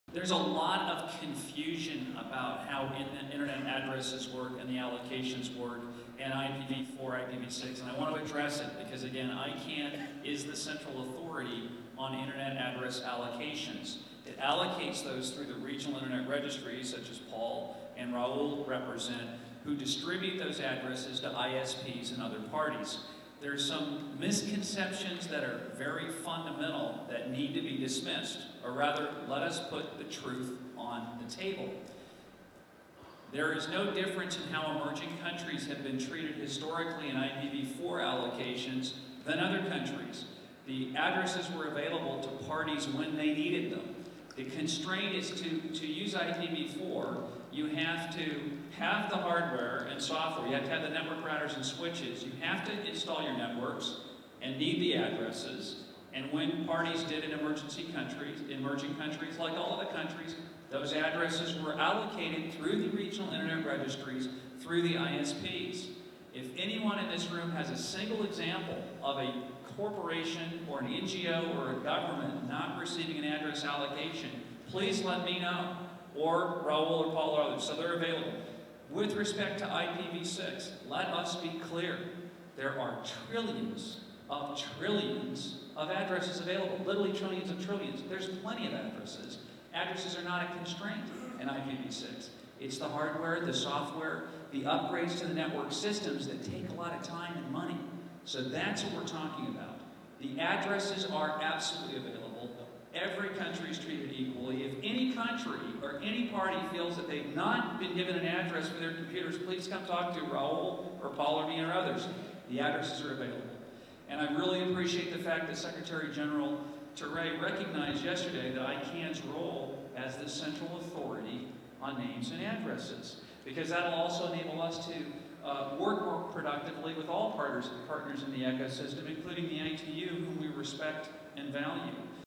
IGF Egypt 2009 Main Session on Managing Critical Internet Resources: Rod Beckstrom Opening
Rod Beckstrom, president/CEO of ICANN, discusses address availability